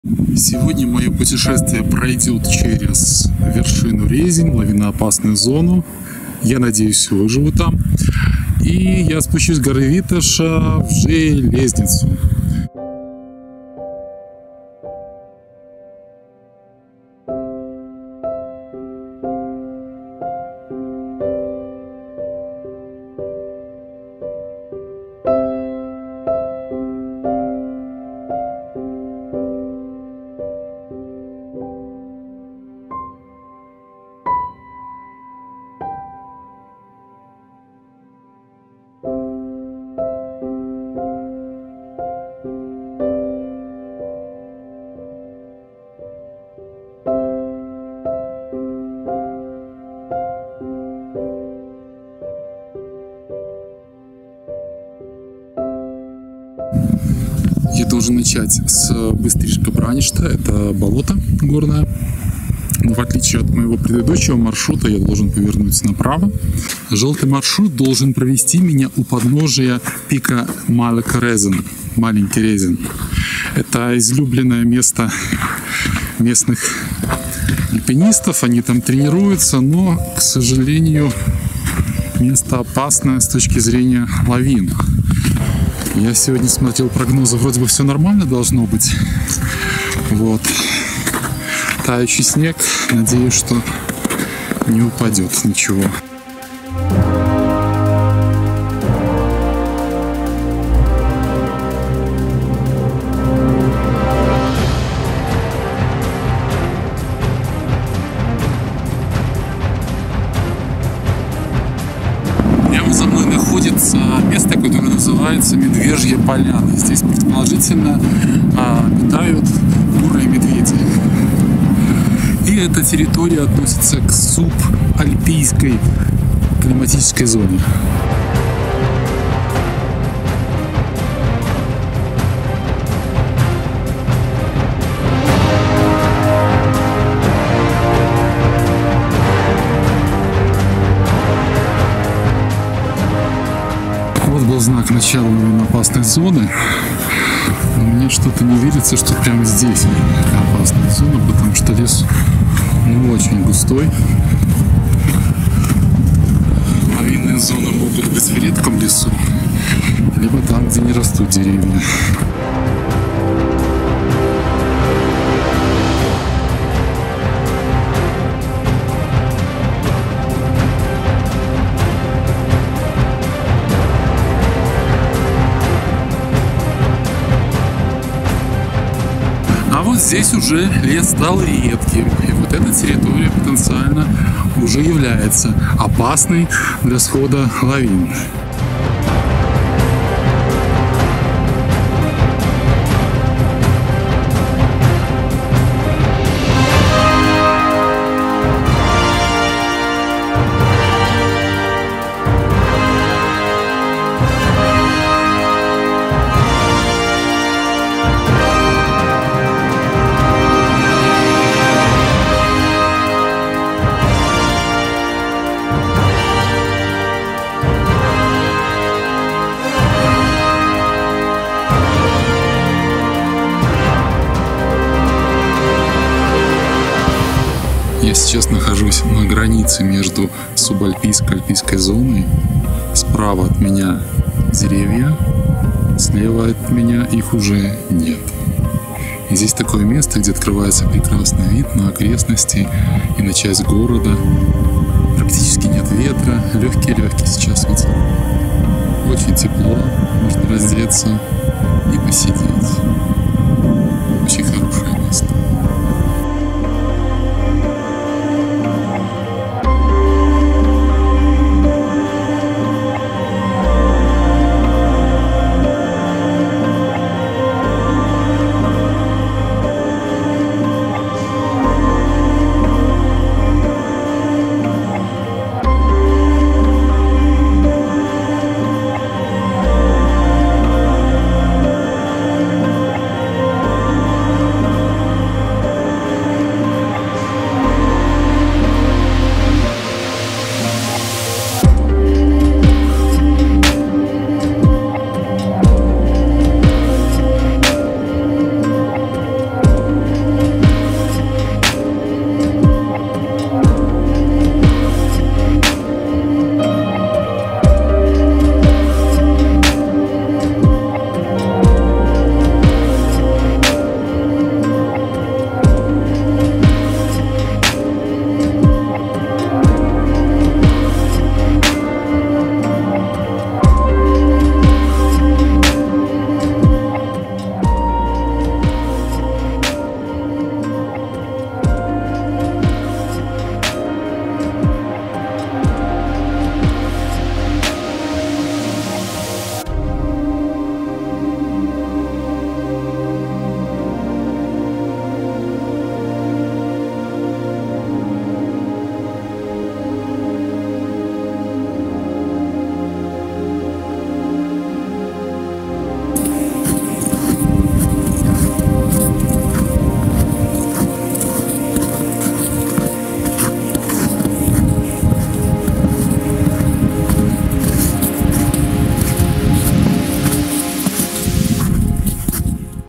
Лавиноопасный Резен - горы Болгарии. 1 серия
Хайкинг на самую красивую тропу горы Витоша.
Северная часть Витоши имеет более крутые склоны, что создает необыкновенной красоты пейзажи.
Тропа проходит у подножия скал Малый и Большой Резен, у крутых заснеженных склонов с глубиной снежного покрова в 1 метр.
Снег спрессован в крепкий ферн, который порой весьма скользкий, а местами проваливается.